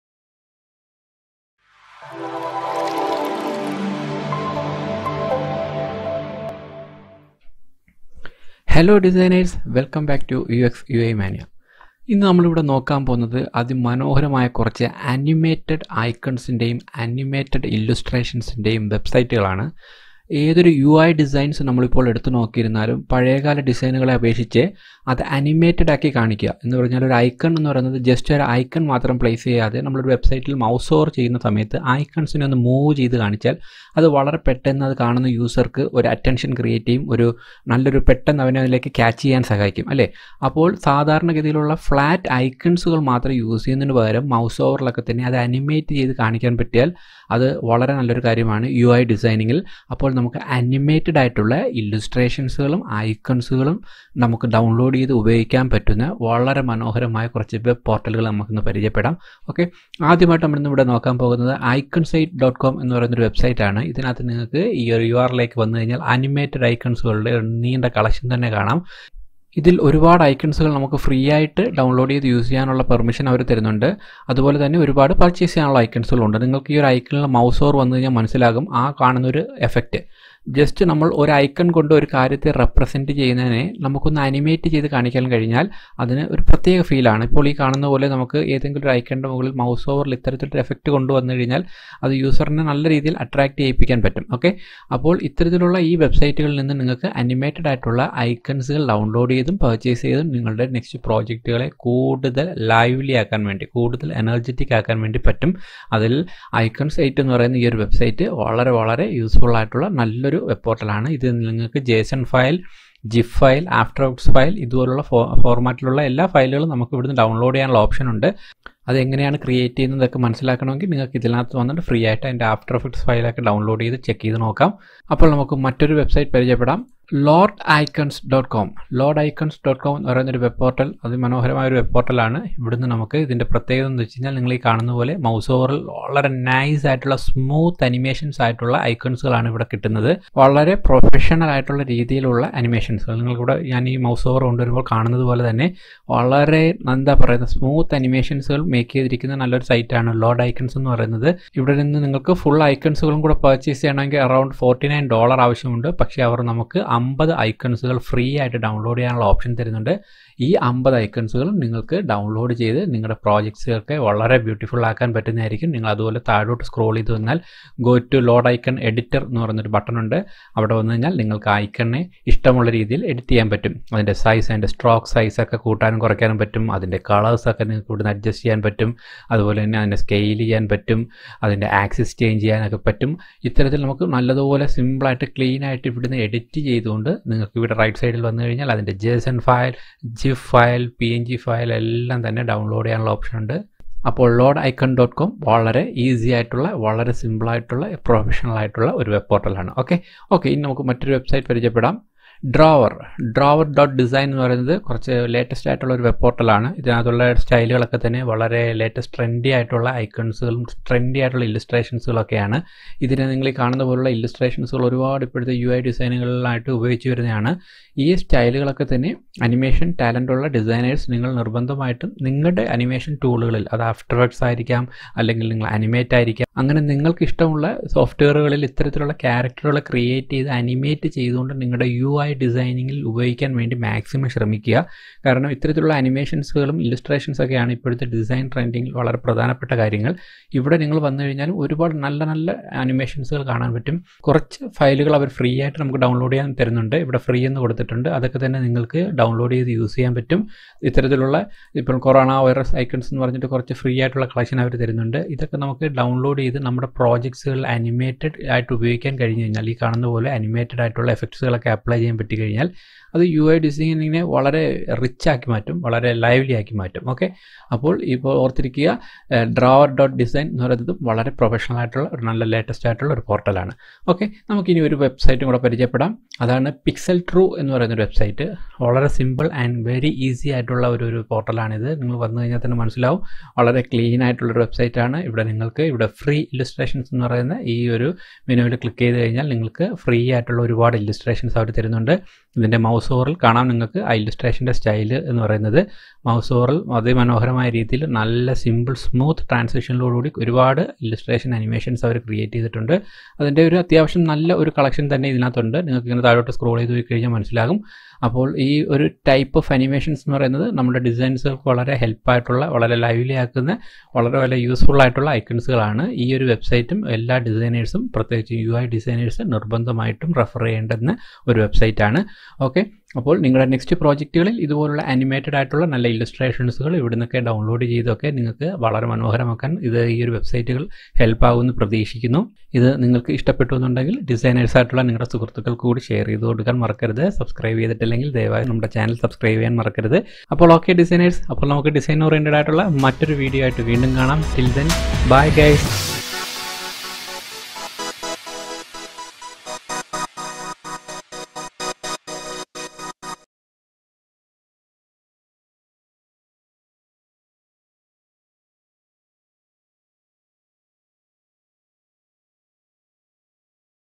Free Animated Illustrations and Icons for designers | UI UX design | Malayalam
Free Animated Illustrations and Icons for designers

Hello Designers,

Let's explore some Free Animated Illustrations and Icons for designers. Please do watch the video and share your feedback, doubts, suggestions, etc. as a comment here. Or a direct message on Instagram.

Thanks,
uxuimania

UI UX design process

Typography Terms

Social Media Post Sizes